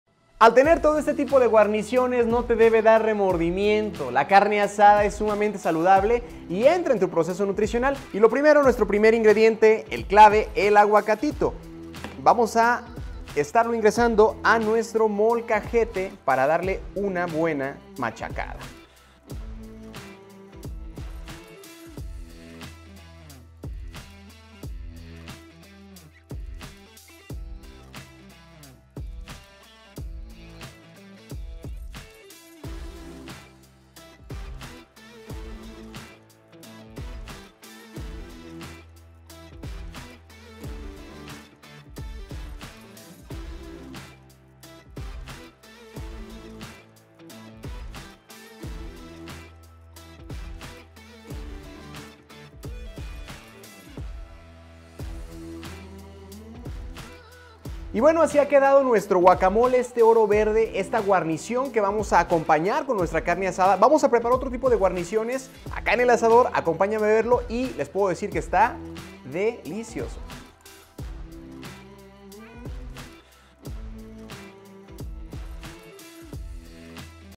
Guarniciones para carne asada
Una buena carne asada comienza desde la cocina, es por ello te traemos la mejor guarnición que tu platillo debe tener si estás en ese proceso